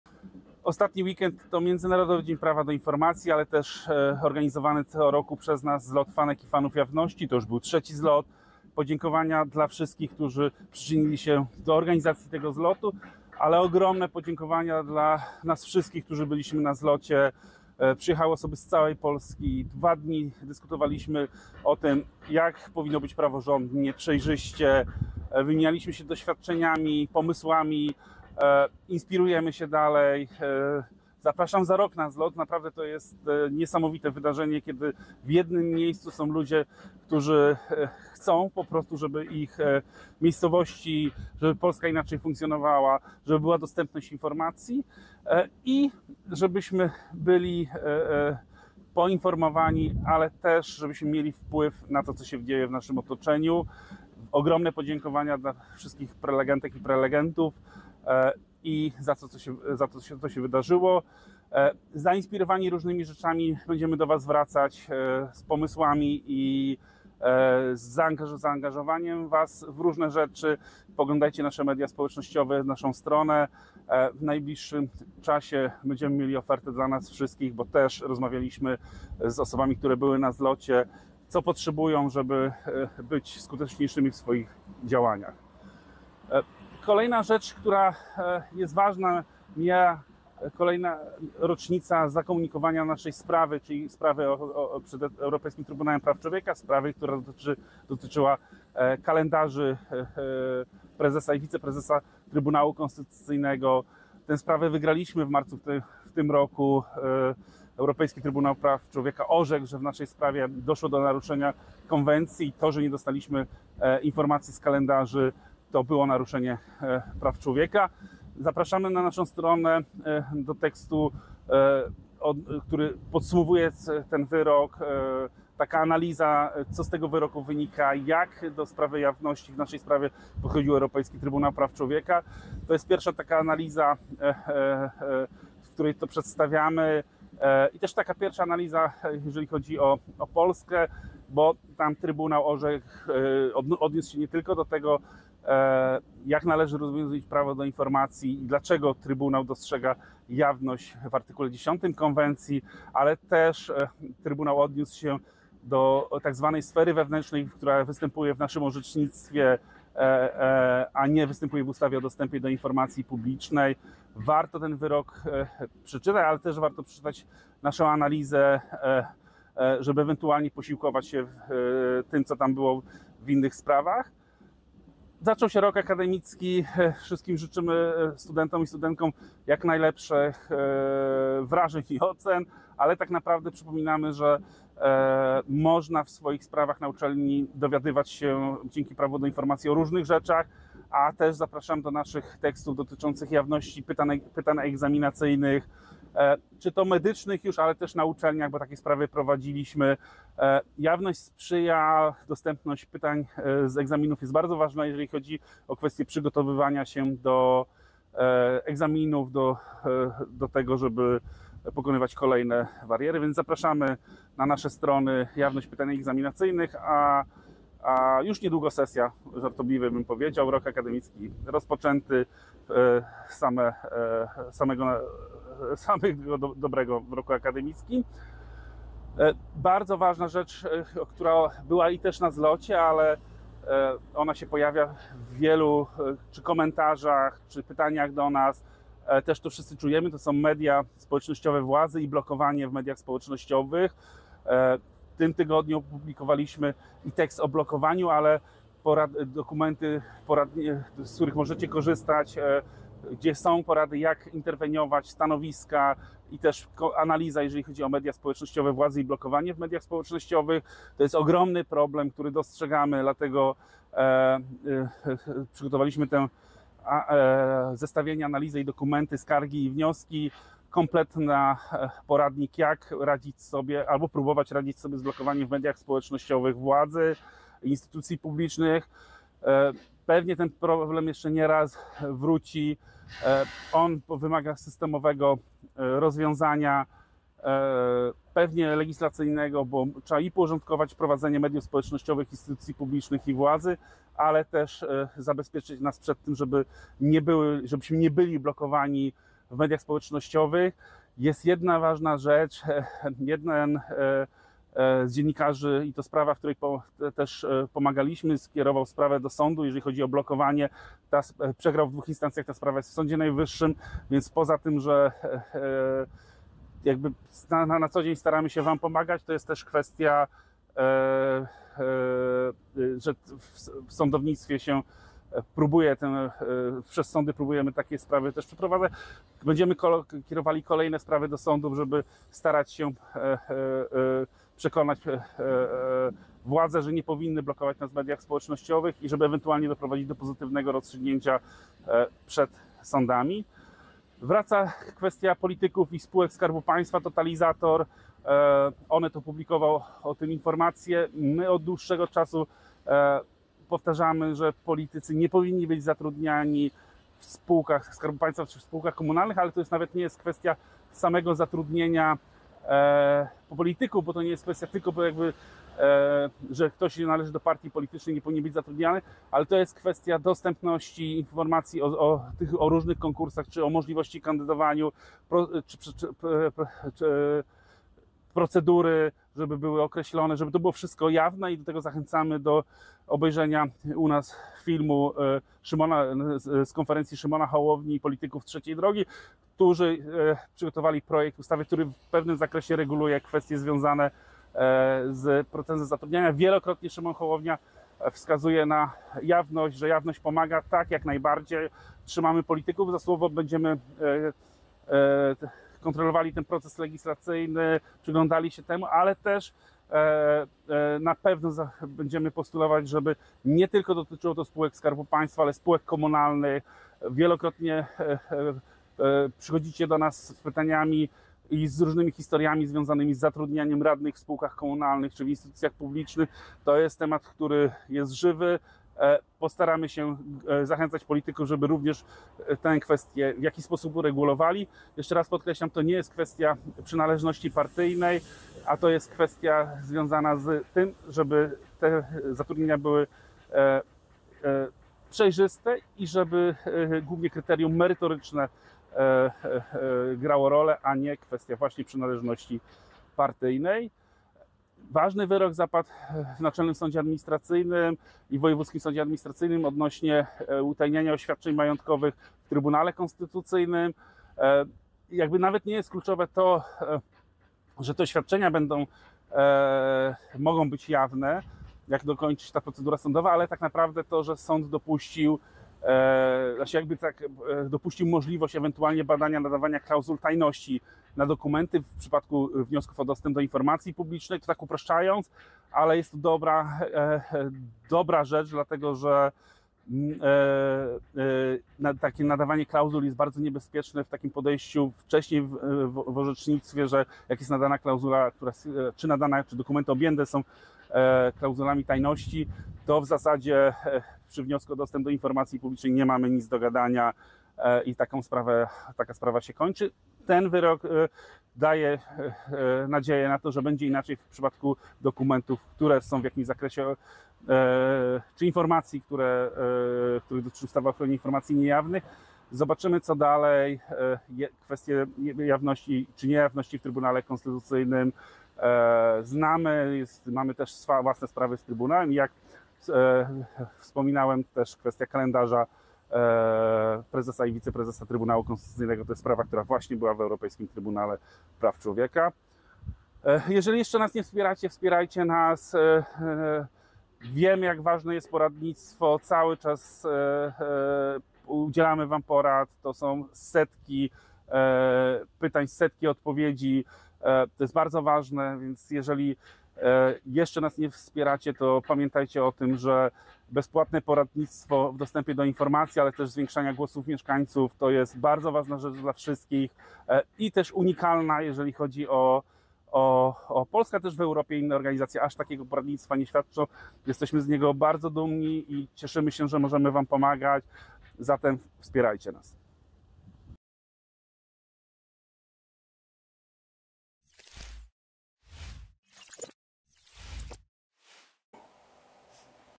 Okiem Watchdoga odc. 38 - 04.10.2024 r.
Ach, co to był za tydzień! W tym odcinku Okiem Watchdoga Szymon Osowski podsumowuje i dziękuje za Zlot Fanów i Fanek Jawności, zachęca do przeczytania analizy wyroku w naszej sprawie przed Europejskim Trybunałem Praw Człowieka, mówi o zatrudnianiu polityków w spółkach oraz poleca Wam tekst i narzędzia, które przygotowaliśmy, by pomóc Wam reagować w przypadkach blokowania w mediach społecznościowych władz i instytucji.

PS Wiemy, że w tle pojawia się też troszkę inny watchdog. :-) Dobrego weekendu!

Przypisy do odcinka:

1. Wnioski z wyroku ETPCz w sprawie Watchdog vs. Polska
2.  Blokowanie w social mediach - analiza, wzory pism
3. Kilka z naszych tekstów o jawności na uczelniach:
4. O wyroku NSA w sprawie oświadczeń majątkowych w TK